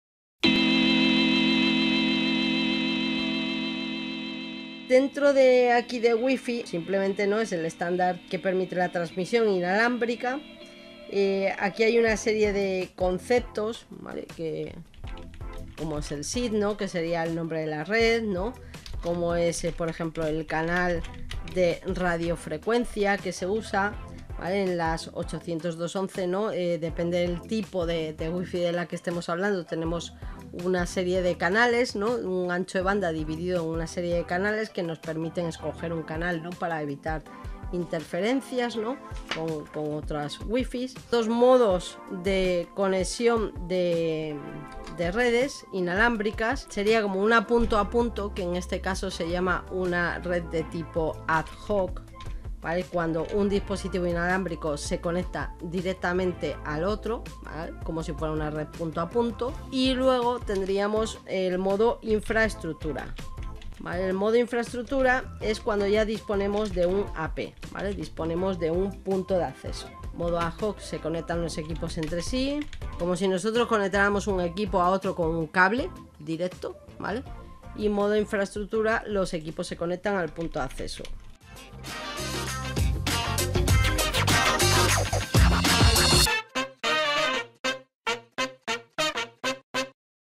REPASANDO REDES 45 Estándares 802 11 WIFI
Redes WIFI, frecuencias, tipos y velocidades.